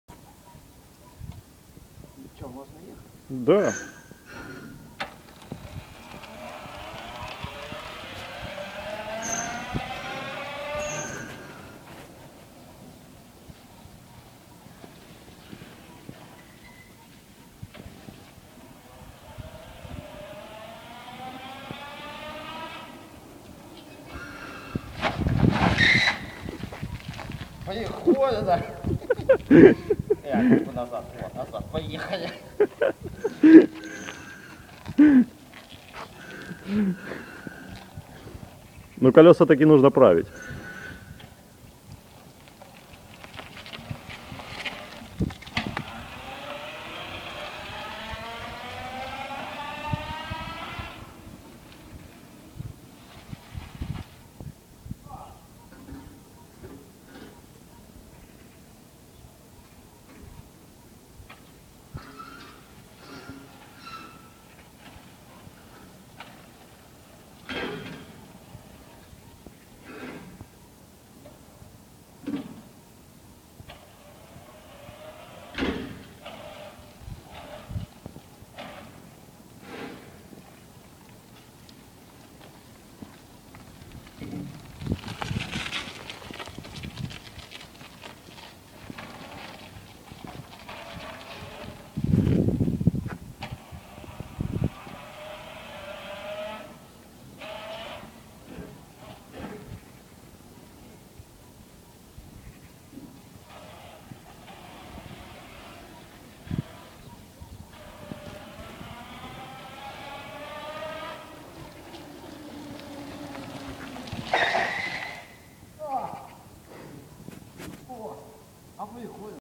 Vugluskr riding ET-1 Electric Trike
My friend Dmitry Bogun aka "Vugluskr" riding our inhouse-builded electric trike.